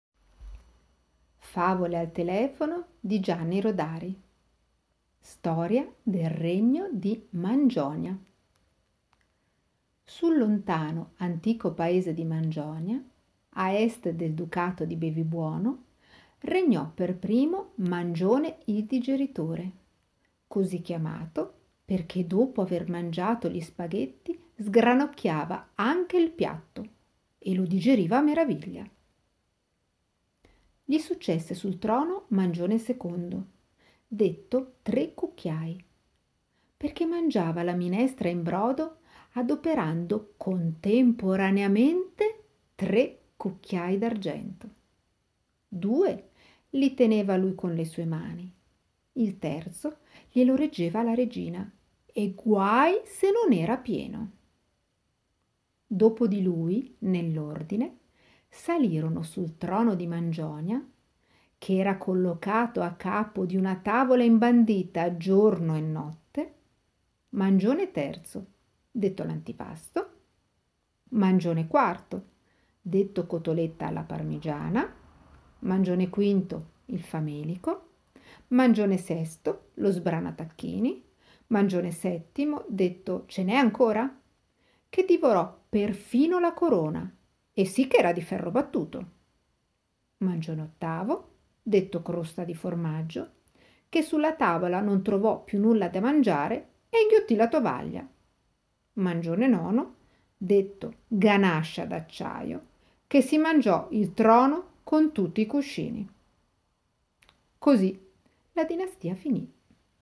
storia del regno di Mangionia - Rodari
Favole al telefono di Gianni Rodari
Voce: Alta Voce - Simona Elettra Lecchi
Comune di Montano Lucino